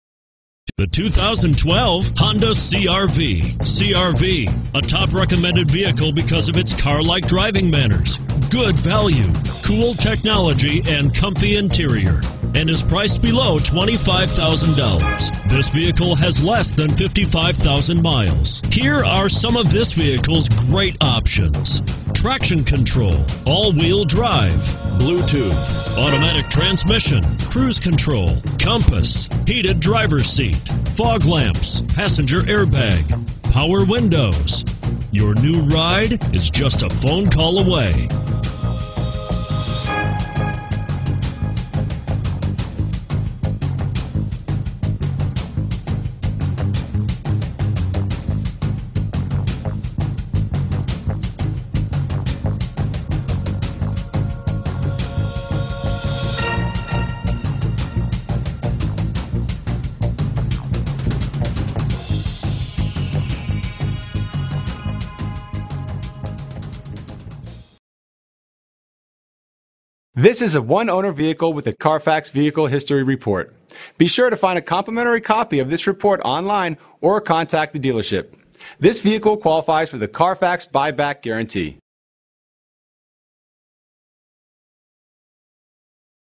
2012 Honda CR-V DC, Maryland, and Virginia 94153
2012 Honda CR-V EX-L - $20595

Don't miss this 2012 Honda CR-V. It's equipped with Automatic transmission and features a Crystal Black Pearl exterior. With 51097 miles, You'll want to take this  home. Make a great choice today.